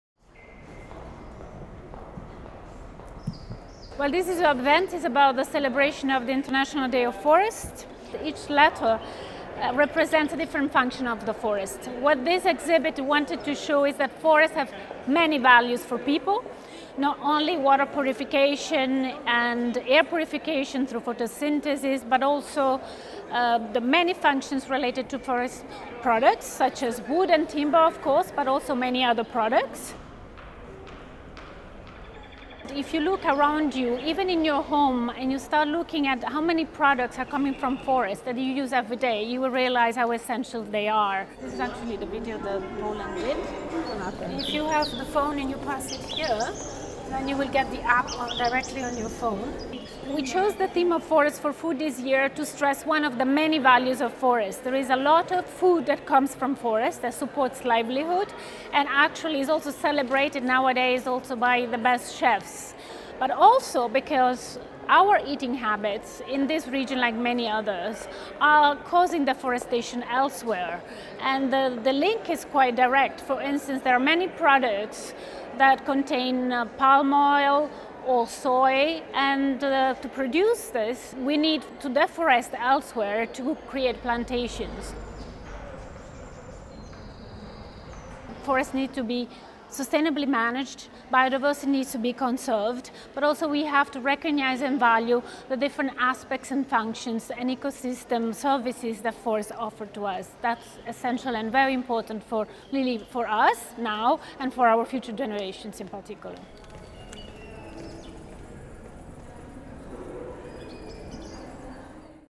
Celebrating International Day of Forests with Food from the Forests at UN Geneva
This year UNOG, UNECE and FAO hosted the annual “International Day of Forests” on 20 March 2015 at the Palais des Nations in Geneva. This year’s celebration was dedicated to the theme of “Forests for Food – Food for Forests”  and drew the attention to the forest’s environmental, cultural, social, economic and gastronomic benefits with a series of events.